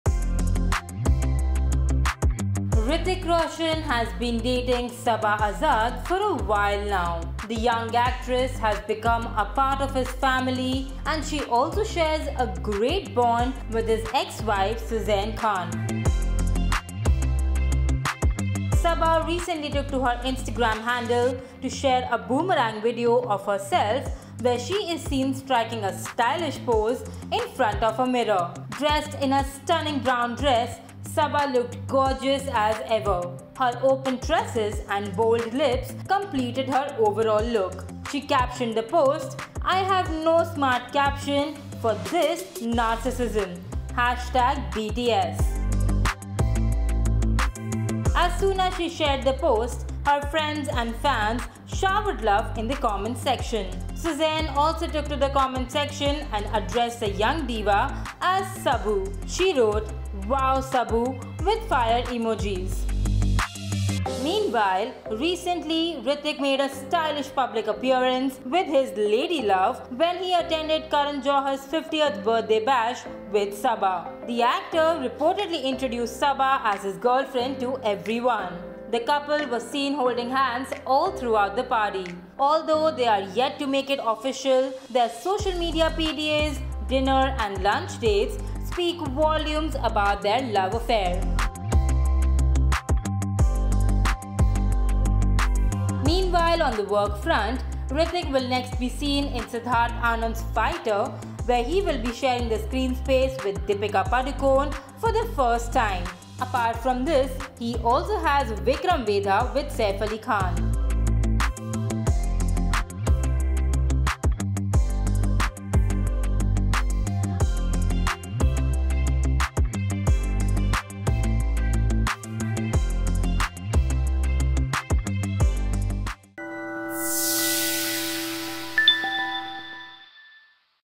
Sussanne Khan has a cute nickname for Hrithik Roshan's girlfriend Saba Azad
Hrithik Roshan has been dating Saba Azad for a while now. The young actress has become a part of his family and she also shares a great bond with his ex-wife Sussanne Khan.
Saba recently took to her Instagram handle to share a boomerang video of herself where she is seen striking a stylish pose in front of a mirror. Sussanne also took to the comment section and addressed the young diva.